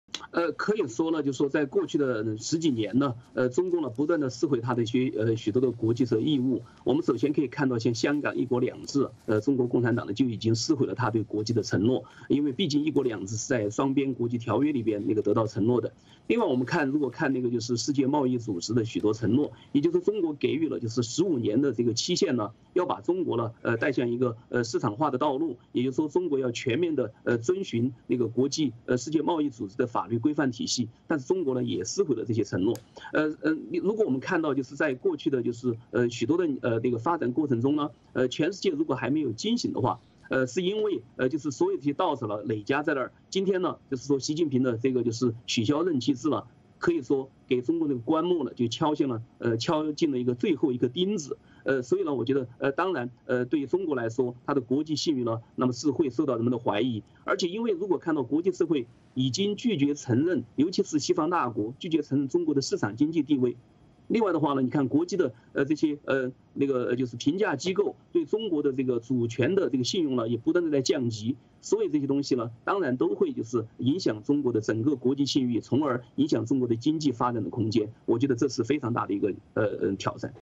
【夏明：取消任期制，中国自挫国际信誉和经济空间】 3/2
夏明：可以说在过去的十几年，中共不断撕毁它国际责任和义务。首先可以看到，香港一国两制，中国共产党就已经撕毁了它对国际的承诺。毕竟一国两制是在双边国际条约里面得到承诺的。世界贸易组织的许多承诺，给予了中国15年的期限，一边走向一个市场化的道路，也就是说中国要全面遵循国际世界贸易组织的法律规范体系。但是中国也撕毁了这些承诺。我们可以看到，在过去的许多发展过程中，全世界如果还没有警醒的话，是因为所有问题累加在那里。今天习近平取消任期制，可以说给中国的棺木敲进了最后一个钉子。对中国来说，它的国际信誉会受到人们的怀疑。因为如果国际社会已经拒绝承认中国的市场经济地位，国际的评价机构对中国主权的信用也在不断的降级。所有这些东西都会影响中国的国际信誉，也会影响中国经济发展的空间。我觉得这是非常大的一个挑战。